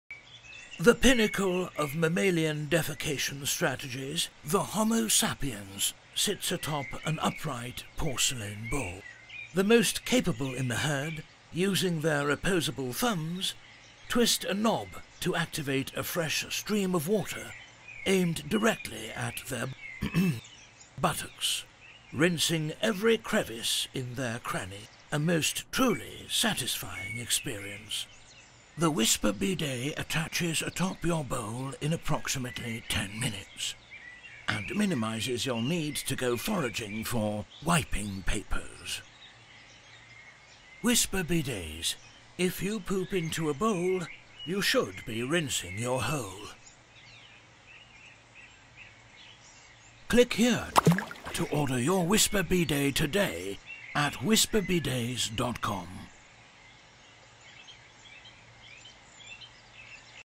If You Poop Into a Bowl Rinse Your Hole - The Whisper Bidet
- Add the Whisper bidet to your toilet in 10 minutes and start rinsing your hole clean today. Cleaning up after a poop never felt better. The Whisper Bidet has been featured on Viceland and has buttloads of happy bathroom-goers.